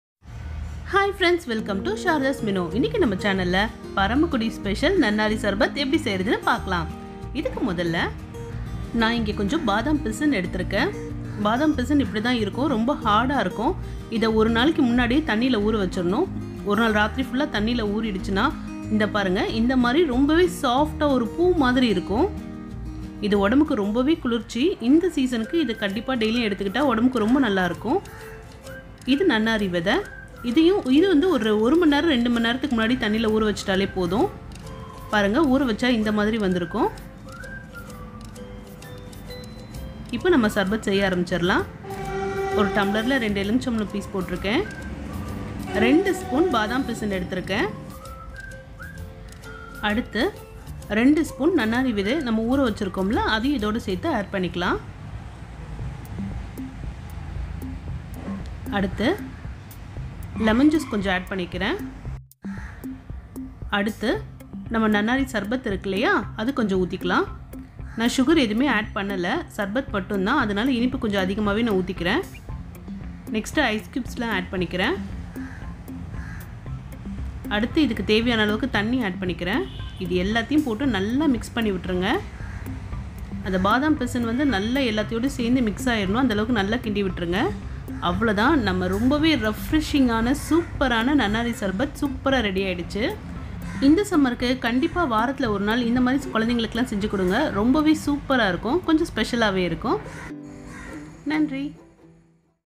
நன்னாரி சர்பத் செய்வது எப்படி/ NANARI SARBATH RECIPE IN TAMIL/ REFRESHING JUICE
Hi friends welcome to Saradha’s menu

Today our recipe is a refreshing drink
It's our native special drink

It's nanarisarbath it's a paramakudi special drink

Let's check out the ingredients for the drink

Lemon juice  - 3tps
Sarbath - 3tps
Batham pisin- 2tps
Sabja seeds - 2tps
Icecubes - needed

With the help of these ingredients let's make the refreshing juice and enjoy  the summer...